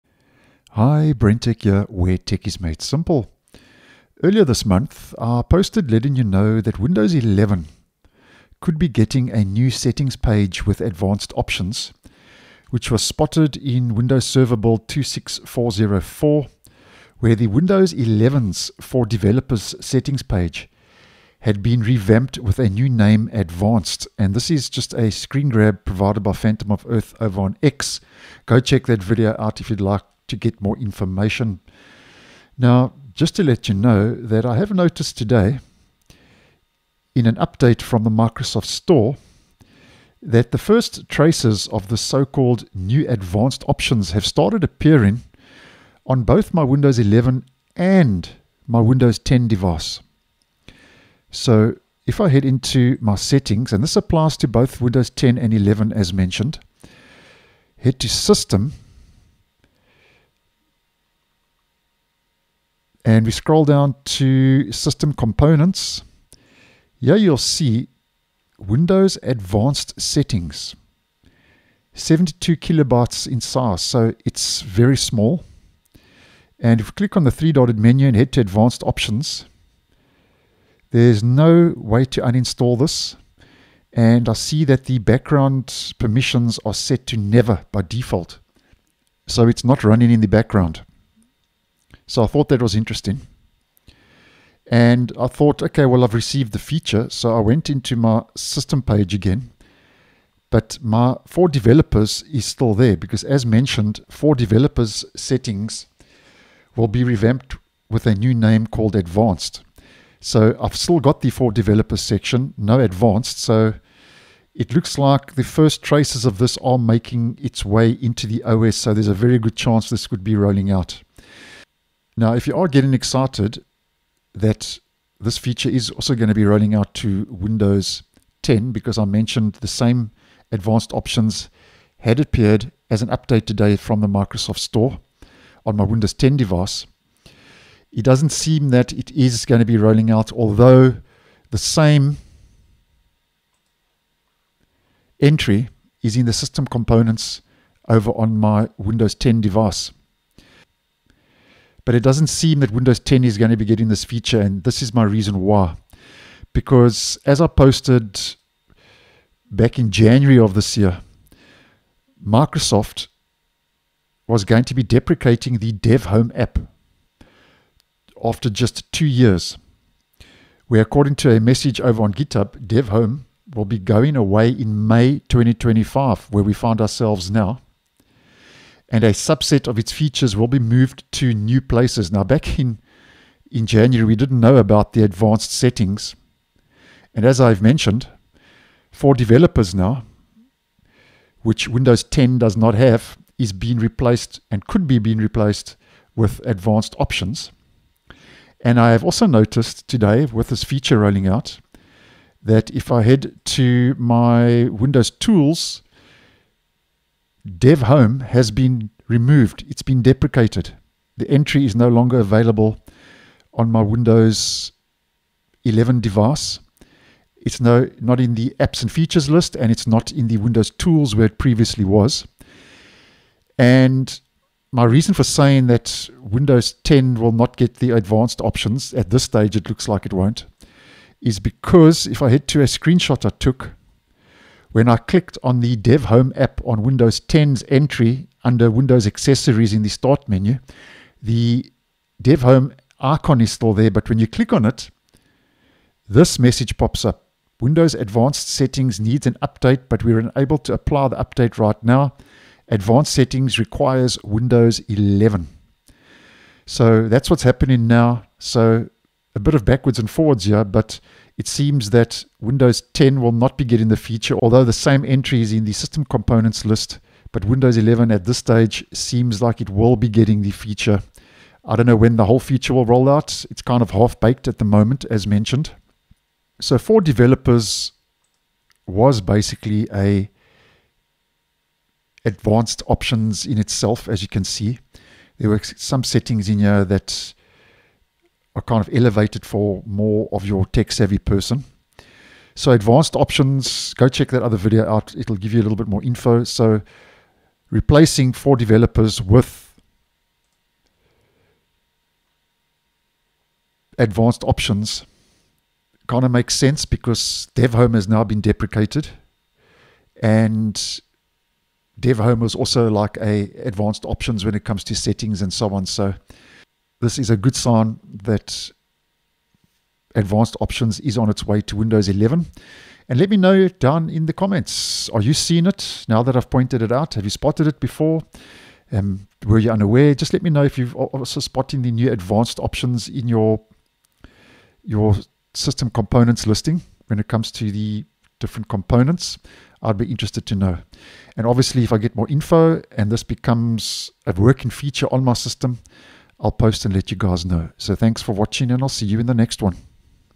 Traces of New Advanced Settings Just Appeared on My Windows After Dev Home's Deprecation!
Spotted a new "Windows Advanced Settings" entry in System Components on Windows 10 and Windows 11 after it was downloaded automatically from the Microsoft Store. Microsoft has now also deprecated the Dev Home app after just 2 years.